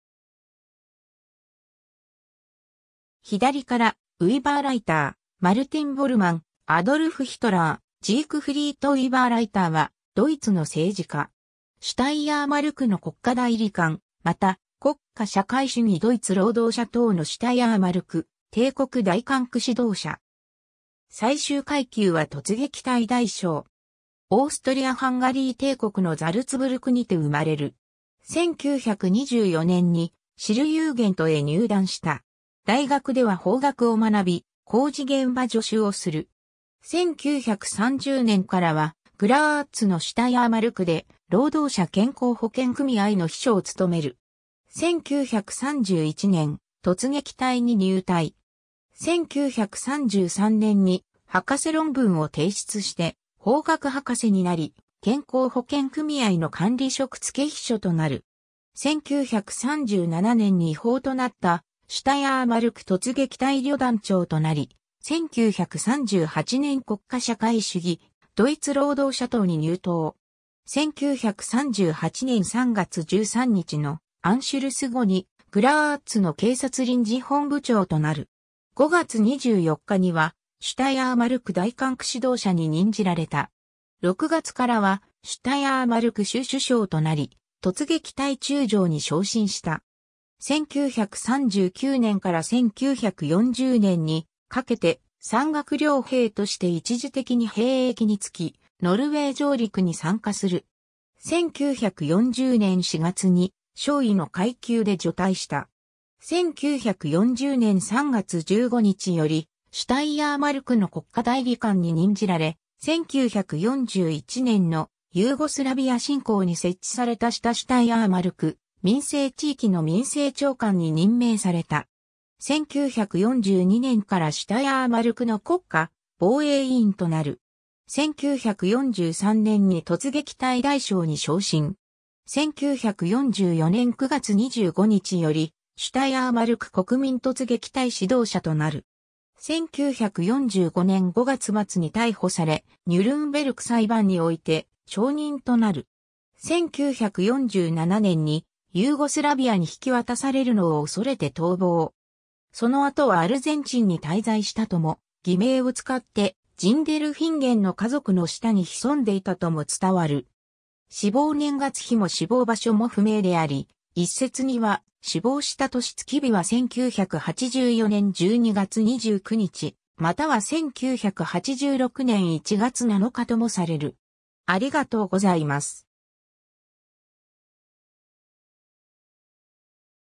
ジークフリート・ウイバーライター
左からウイバーライター, マルティン・ボルマン, アドルフ・ヒトラー 、 ジークフリート・ウイバーライター（Siegfried Uiberreither、1908年3月29日- 1984年12月29日?）は、ドイツの政治家。
シュタイアーマルクの国家代理官(de:Reichsstatthalter)、また国家社会主義ドイツ労働者党のシュタイアーマルク帝国大管区指導者。
最終階級は突撃隊大将。
 オーストリア＝ハンガリー帝国のザルツブルクにて生まれる。
1924年に、シルユーゲント（Schilljugend）へ入団した。
大学では法学を学び、工事現場助手をする。
1930年からはグラーツのシュタイアーマルクで労働者健康保険組合の秘書を務める。
 1931年、突撃隊に入隊。
1933年に博士論文を提出して法学博士になり、健康保険組合の管理職付秘書となる。
1937年に違法となったシュタイアーマルク突撃隊旅団長となり、1938年国家社会主義ドイツ労働者党に入党（党員番号6,102,560）。
1938年3月13日のアンシュルス（オーストリア併合）後にグラーツの警察臨時本部長となる。
5月24日にはシュタイアーマルク大管区指導者に任じられた。
6月からはシュタイアーマルク州首相となり、突撃隊中将に昇進した。
 1939年から1940年にかけて山岳猟兵として一時的に兵役につき、ノルウェー上陸に参加する。
1940年4月に少尉の階級で除隊した。
 1940年3月15日よりシュタイアーマルクの国家代理官(de:Reichsstatthalter)に任じられ、1941年のユーゴスラビア侵攻に設置された下シュタイアーマルク民政地域（de）の民政長官に任命された。
1942年からシュタイアーマルクの国家防衛委員（de:Reichsverteidigungskommissar）となる。
1943年に突撃隊大将に昇進。
1944年9月25日よりシュタイアーマルク国民突撃隊指導者となる。
 1945年5月末に逮捕され、ニュルンベルク裁判において証人となる。
1947年にユーゴスラビアに引き渡されるのを恐れて逃亡。
その後はアルゼンチンに滞在したとも、偽名を使ってジンデルフィンゲンの家族の下に潜んでいたとも伝わる。
死亡年月日も死亡場所も不明であり、一説には死亡した年月日は1984年12月29日または1986年1月7日ともされる。